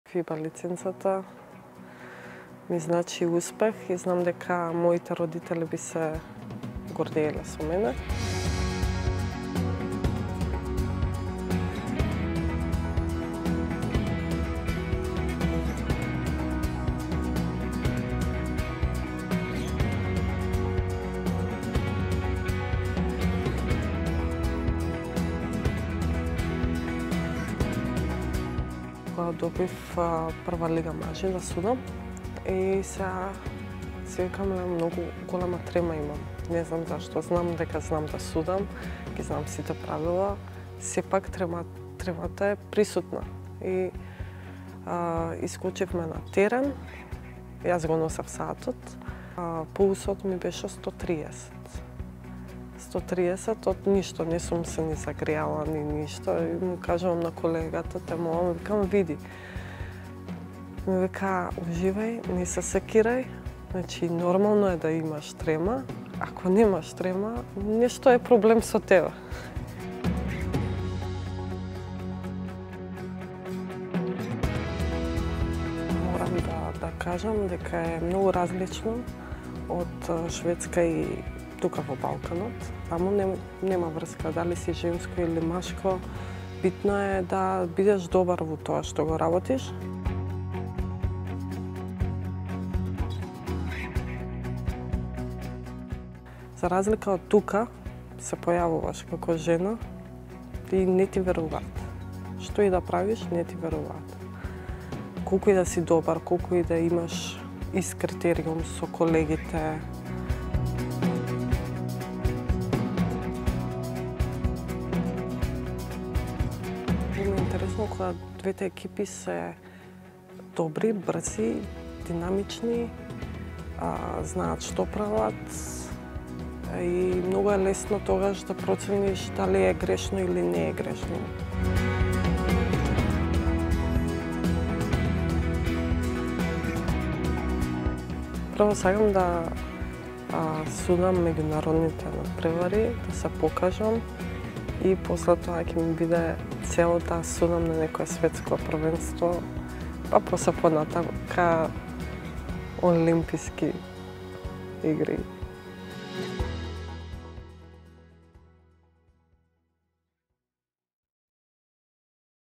Првата ФИБА лиценцирана кошаркарска судијка од Македонија
Погледнете ја приказната за Ема Никочевиќ, првата судијка во кошарка од Македонија која доби ФИБА лиценца.